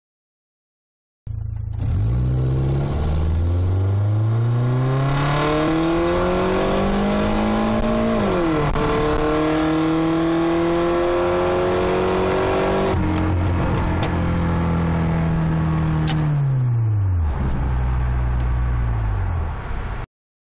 Stock s13 240sx 0-65
This is a 1991 s13 240sx with stock ka24de with fart cannon and k and n box filter in and its a auto...last drive before i sold it to pick up a 5 speed 240sx...videos coming soon (: